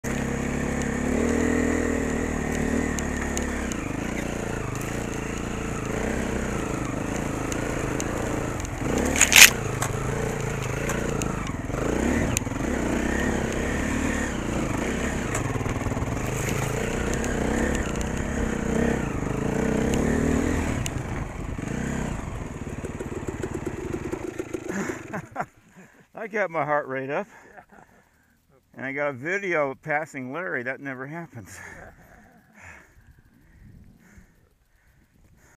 2021_02_16 Following Gary and passing Larry
Following Larry (76) and Gary (75) north of Morristown AZ heading to "Top of the World' trail